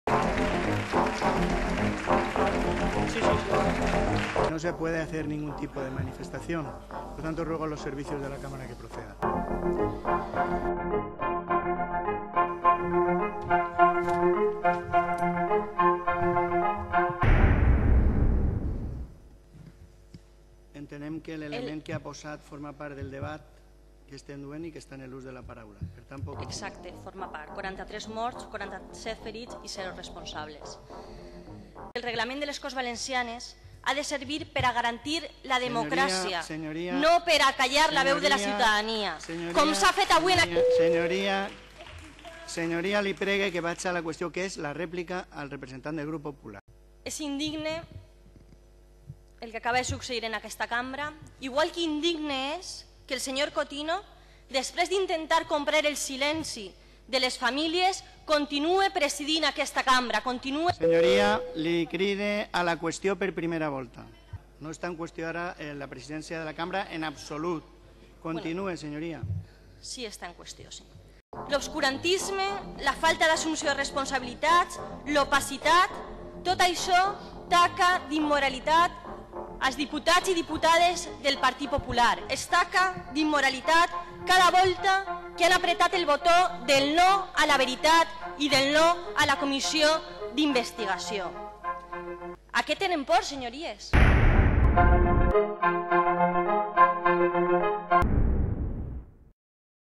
"A què tenen por, senyories?" (El PP torna a censurar la investigació de l'accident de metro)
Després que el president de Les Corts expulsara de l'hemicicle a un membre de l'associació de víctimes del metro de València, la diputada d'Esquerra Unida Marina Albiol exhibeix en la trona una camiseta per a homenatjar les víctimes i denunciar l'enèsima censura del Grup Parlamentari Popular a investigar l'accident i, si cal, depurar responsabilitats polítiques.